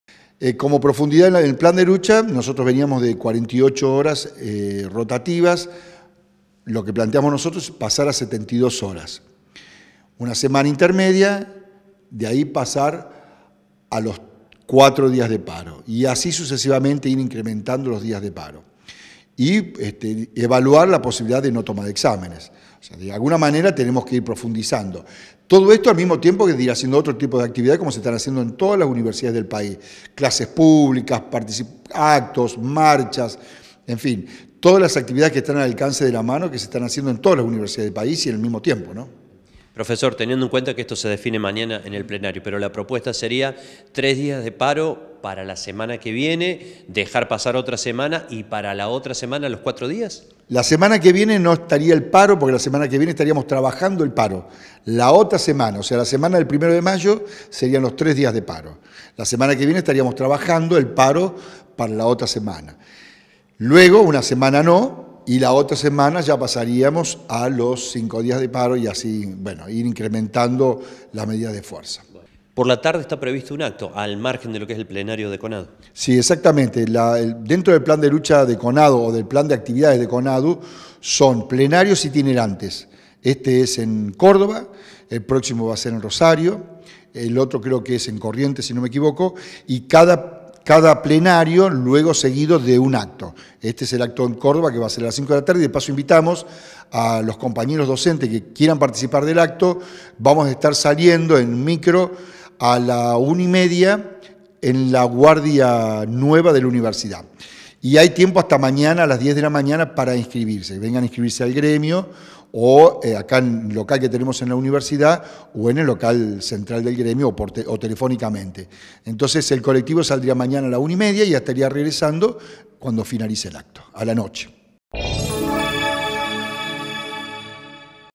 PROFUNDIZACIÓN DEL PLAN DE LUCHA - GUILLERMO ASHWORTH – SECRETARIO GENERAL AGD
ES EL MANDATO QUE SE LLEVA MAÑANA A PLENARIO DE CONADU.

Facebook.com/uniriotv
Twitter.com/uniriotv
Instagram.com/uniriotv
Web - uniriotv.unrc.edu.ar
o envianos un email a
UniRio TV
Universidad Nacional de Río Cuarto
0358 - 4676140